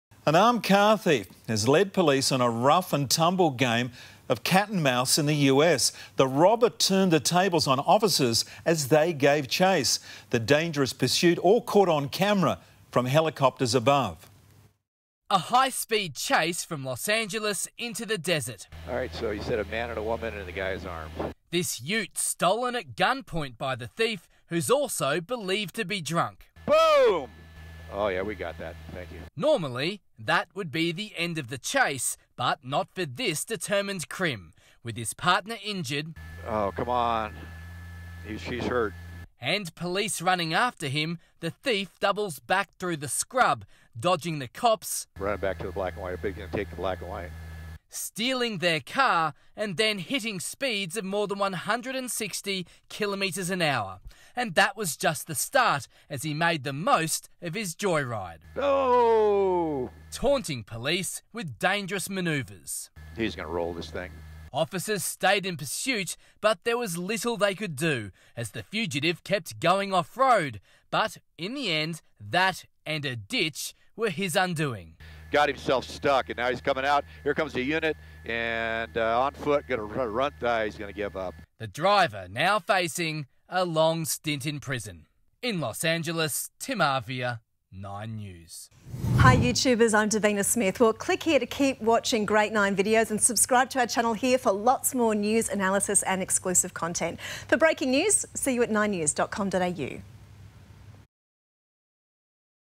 High speed police pursuit through LA caught on camera | Nine News Australia
An armed car thief has lead U.S. police on a dangerous pursuit.